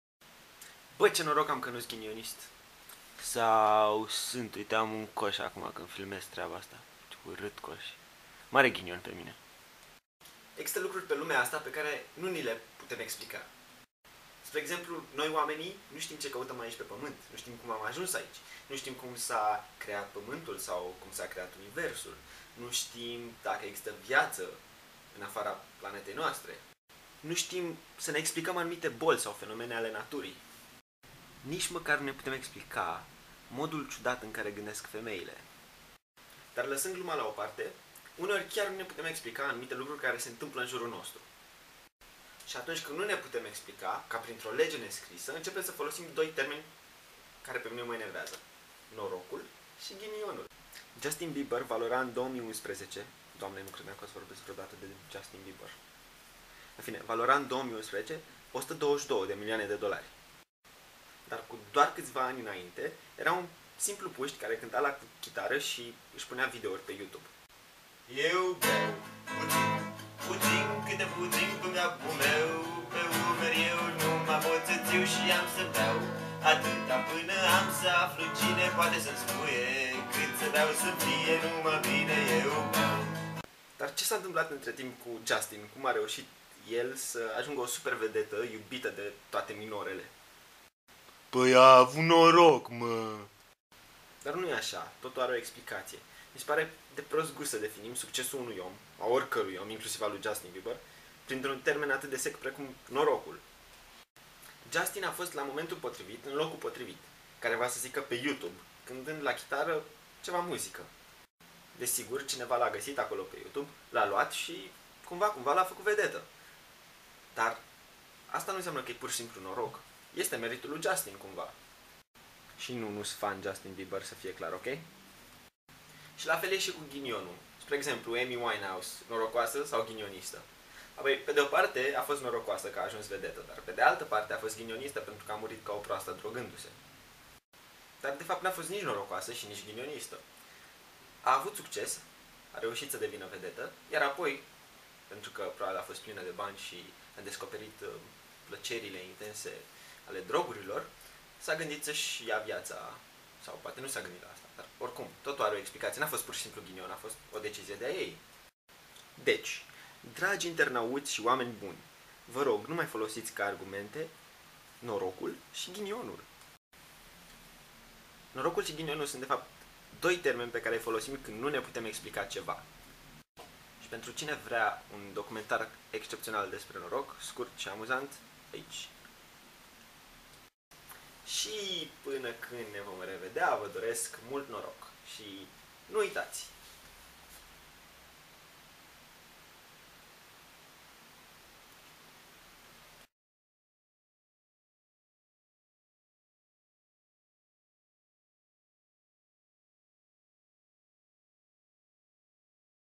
Ep. 6 - Ce noroc am că nu-s ghinionist!
Justin Bieber valora în 2011 aproximativ 122 de milioane de dolari! Cu doar câţiva ani înainte era un puşti cu un cont de YouTube, un anonim, care zdrăngănea la chitară...

Ce s-a întâmplat între timp? Cum a ajuns enervantul Justin să fie o super-vedetă iubită de toate minorele? "A avut noroc!" vei răspunde... DAR nu, nu e aşa!

*să mulţumim berii Noroc pentru că nu s-a întâmplat niciun ghinion în timpul filmărilor...*

Textul complet al acestui vlog poate fi gasit pe blogul meu personal aici: